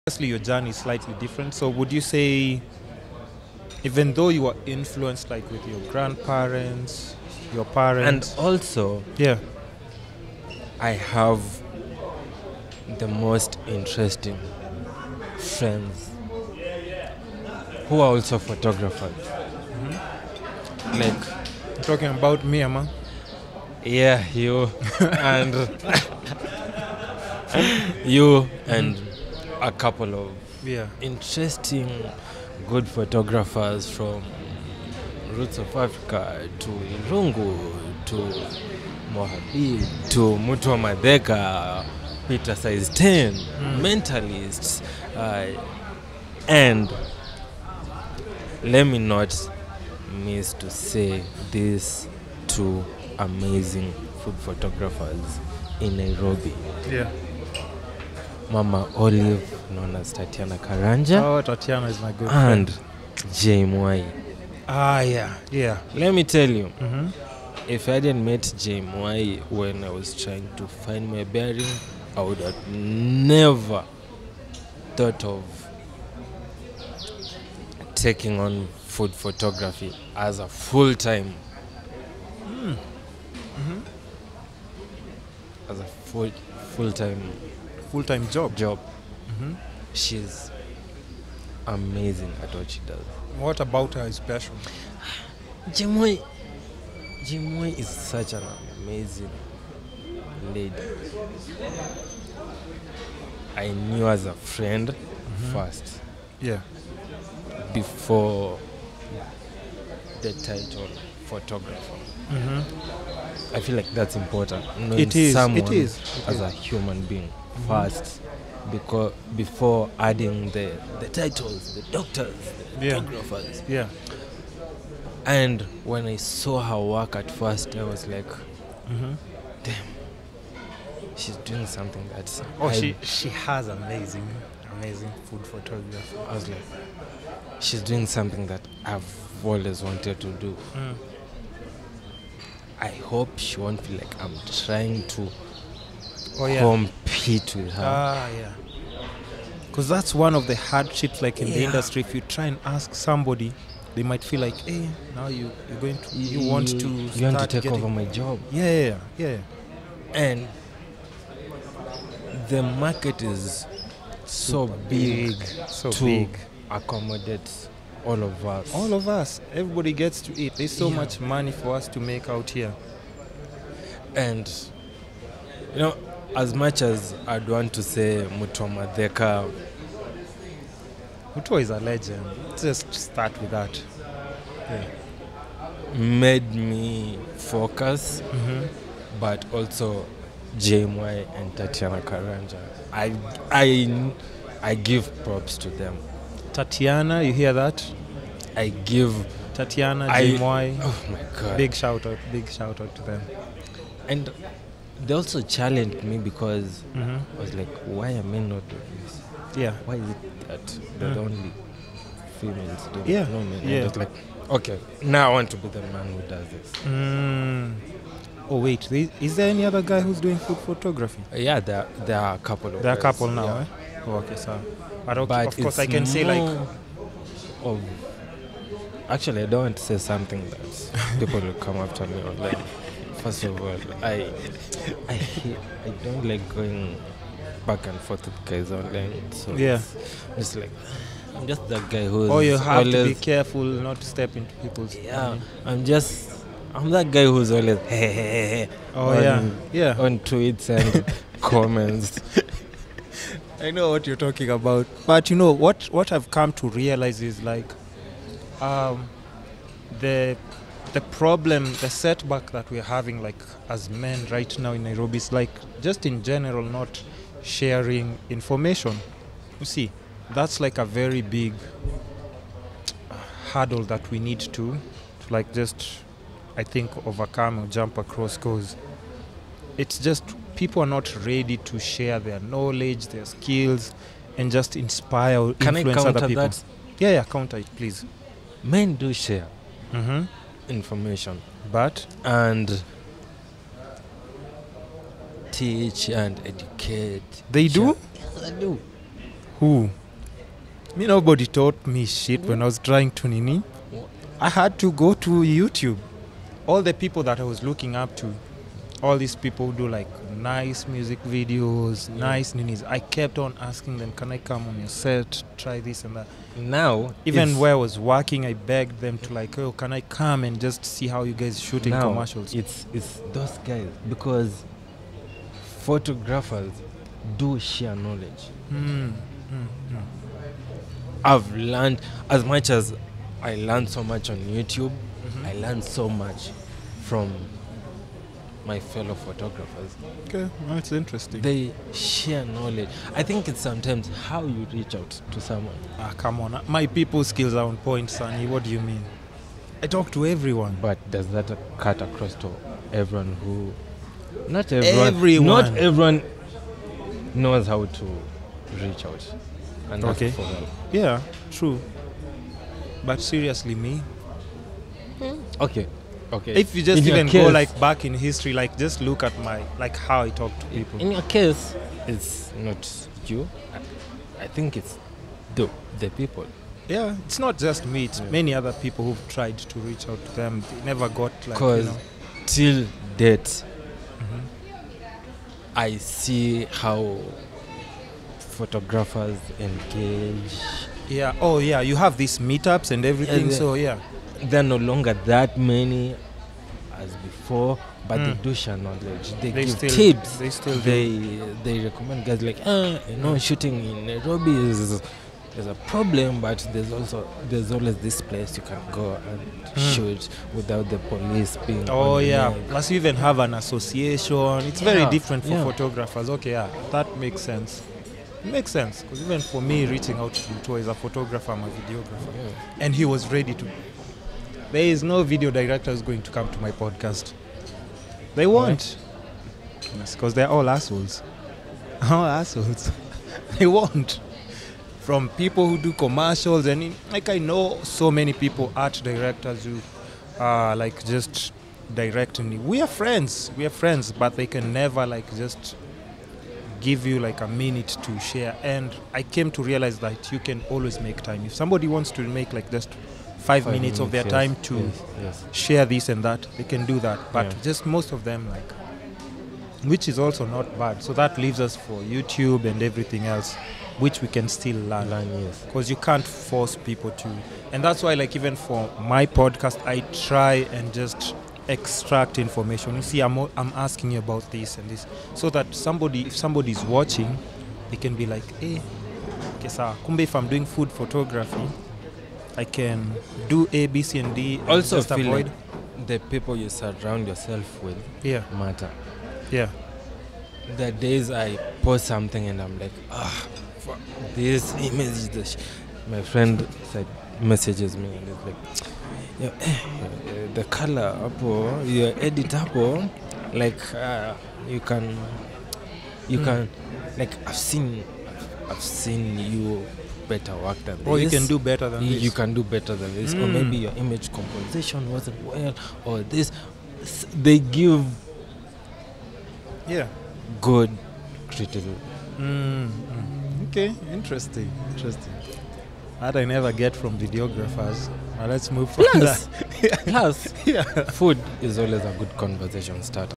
Sannie Sanford Credits Kenya’s finest Photographers Mama Olive, J. Mwai & Mutua Matheka
CAN A MAN GET SOME HELP?! 🤷🏾‍♂️: Sannie Sanford & The Kissing Bandit discuss the struggles of asking for help as a man and the challenges of finding male mentors in the creative industry.

He also gives credit to some of Kenya’s finest photographers Mama Olive J. Mwai Photography & Mutua Matheka who were influential and helped him get his start in food photography.